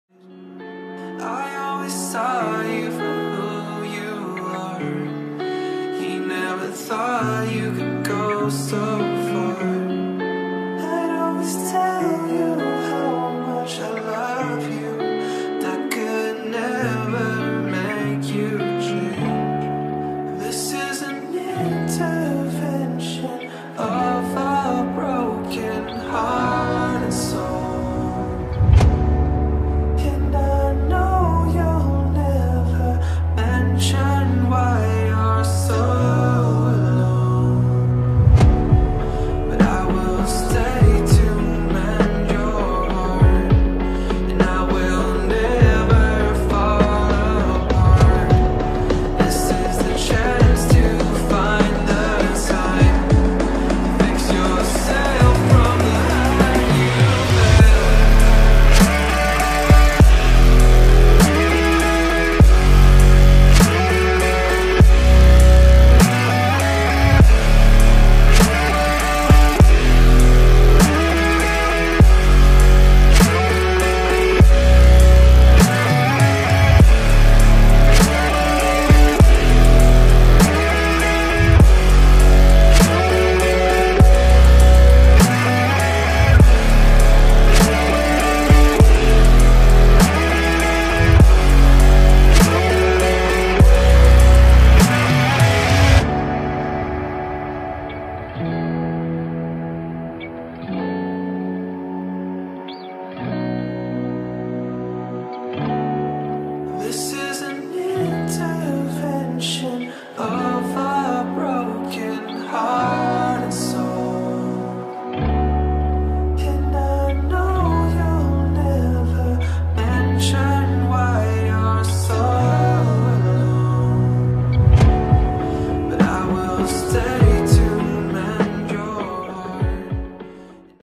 •Re-Drawing• [2018 ART TO 2019] Speed Paint (1)
This is a remake of a drawing I made a year ago I think I’ve been proven a lot lol